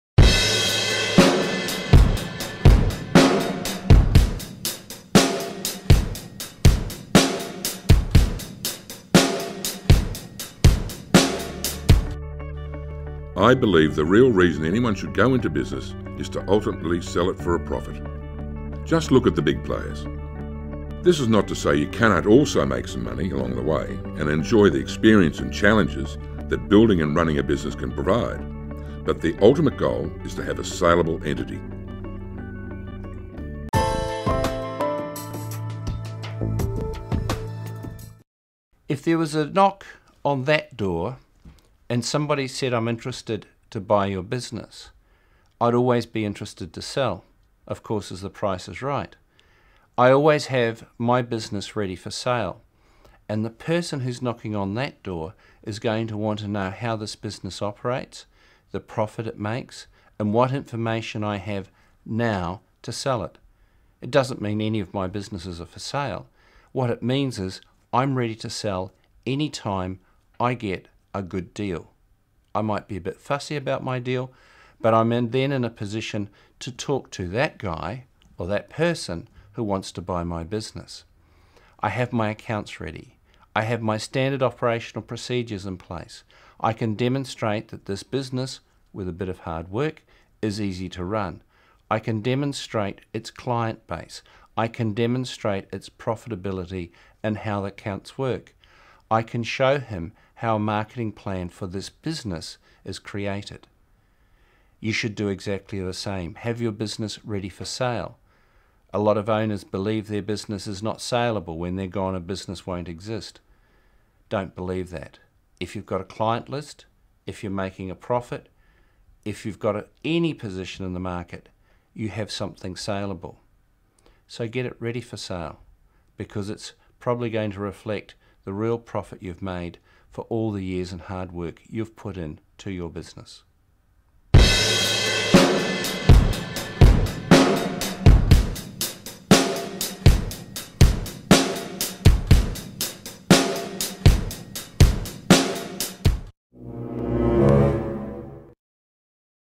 Chapter 22 - Always Have Your Business Ready for Sale
"I believe the real reason anyone should go into business is to ultimately sell it for a profit. Just look at the big players. This is not to say that you cannot also make some money along the way and enjoy the experiences and challenges that building and running a business provide, but the ultimate goal is to have a saleable entity."

This is an extract from the book 'How Not To Commit Business Suicide' by Gavin Waring. Or go to amazon.com.au